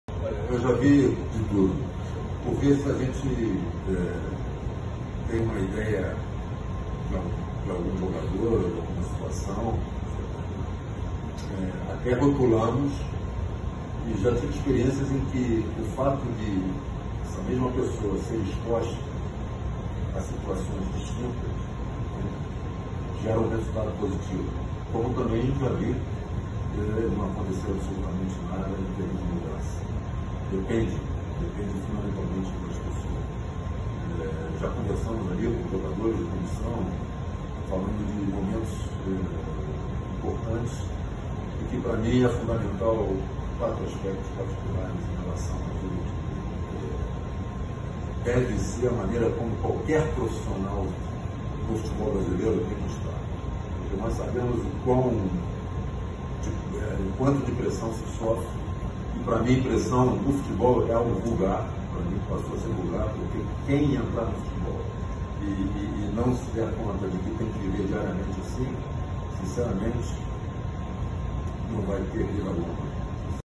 TRECHO DA ENTREVISTA COLETIVA DE PAULO AUTUORI
Primeira entrevista coletiva de Paulo Autuori como diretor de futebol do clube do povo.